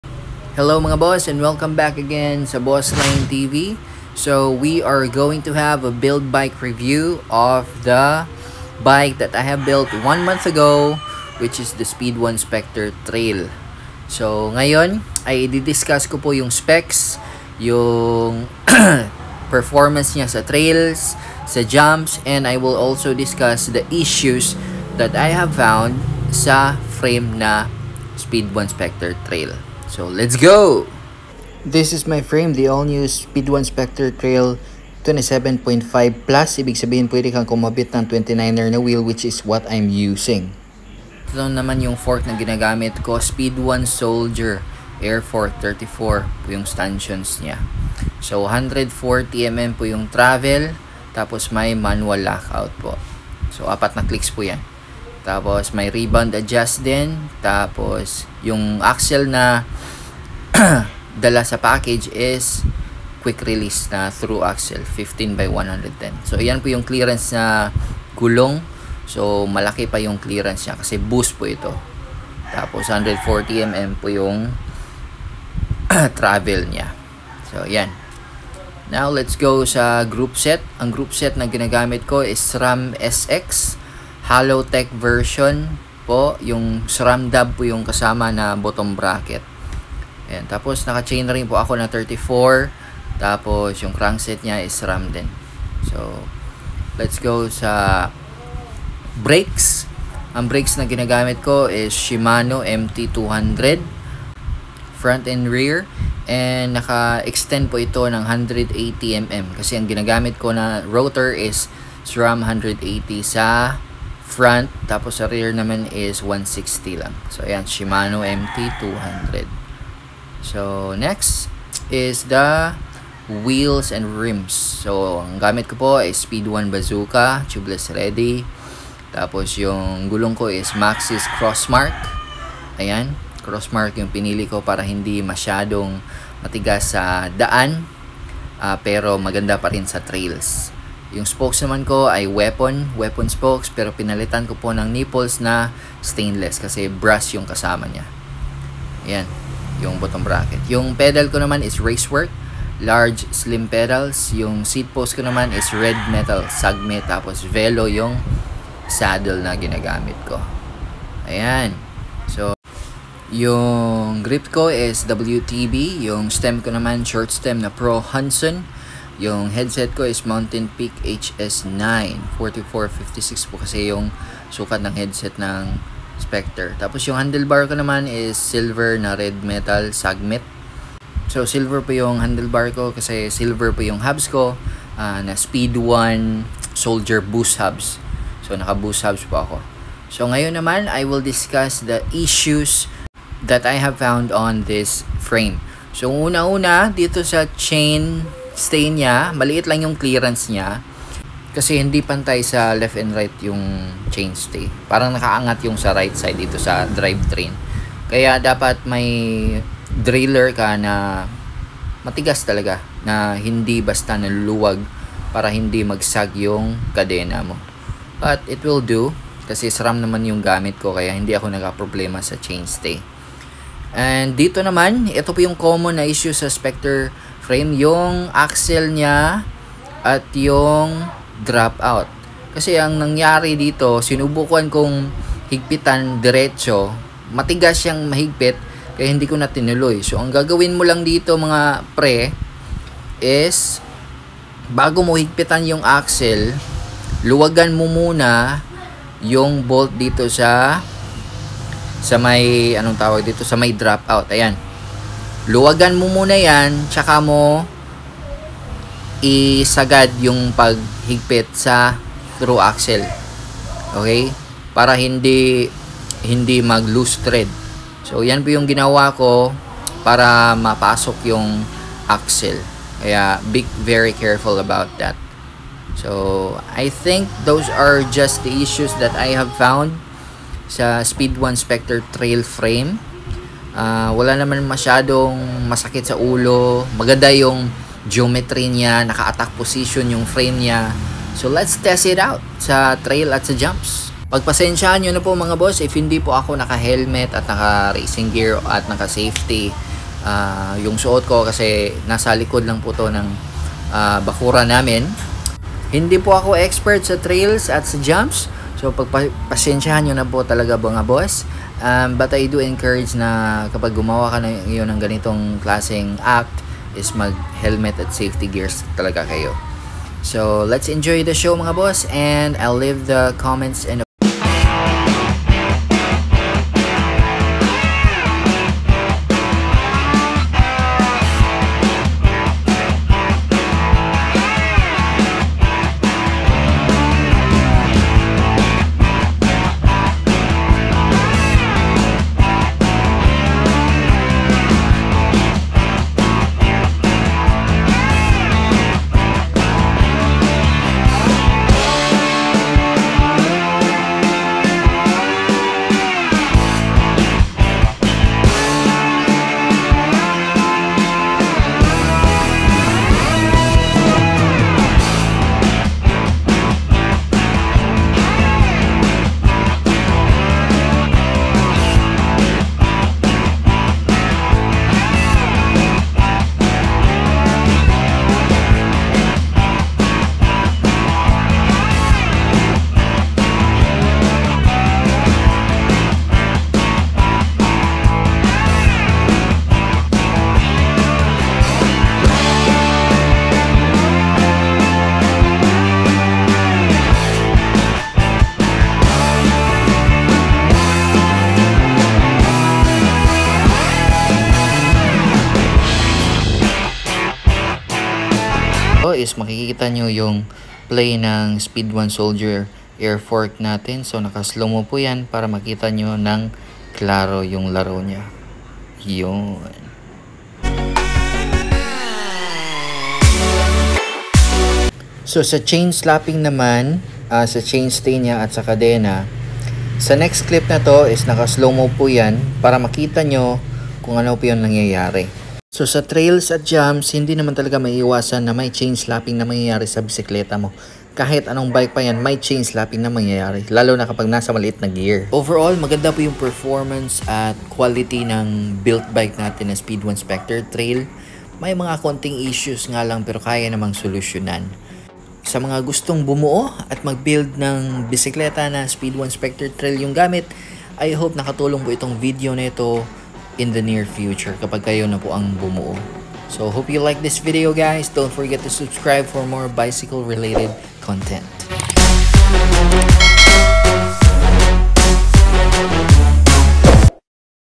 Speedone Spectre Trail Build Bike Review, Trail, Jump Test and honest Issues Revealed 🔥🔥
Paubaya napo at hindi ako naka helmet at naka safety gear since nasa likoran lang ito ng bakuran namin at tsaka nagdahan2x lng ako but still recommended na dapat mag helmet palage habang nagbibike po kayo. Wag tularan yung sa video ☺️

It took some time for me to give a review and test out this bike build of mine since I was busy with personal matters and my new born baby. Well better late than never and here it is mga boss. Based on my experience riding and testing out this bike was great. The frame is good and strong with an aggressive geometry and slack head tube which is good for cross country and trail riding. Hope you liked this video mga boss and please do leave out your own opinion in the comment sections :)

For those who wants to build this kind of bike. These are the parts insalled.

SPECS AND PARTS INSTALLED:
FRAME: Speedone Spectre Trail XC Boost 27.5+ TA
FORK: Speedone Soldier Boost TA 34 140mm Airfork w/ Rebound Adjust + Manual Lockout
Groupset: Straight Sram SX Dub Version
Brakset: Shimano MT200
Rotors: 180mm front 160mm back all SRAM
Wheelset: Speedone Bazooka Rims 29er
                   Weapon Spokes Stainless + Stainless Nipples
                   Maxxis Crossmark II 29x2.25
                   Speedone Soldiers Boost Hubs
                   Black Cat Tubes
Cockpit: Sagmit RedMetal Straight + Backsweep Handle Bar 760mm
                Sagmit RedMetal Seatpost 31.6mm
                ProHonsun Short Stem
PEDAL: Racework Wide Pedals
OVERALL WEIGHT: 13.62 KGS

Thank guys for watching and see you again next time ‼️
RIDE SAFE AND GODBLESS 🙏